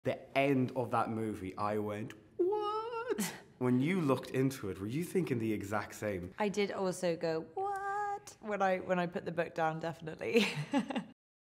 WHHHHAAAAAAAATTTTT!!! 😂 Daisy Edgar Jones shared in our disbelief 🙌
Crossy sat down for a chat with the always lovely Daisy Edgar Jones to chat all things Where the Crawdads Sing. The pair talk jumping into cold bodies of water, doing stunt work for the first time, what it's like to work with Reese Witherspoon and getting use to fame.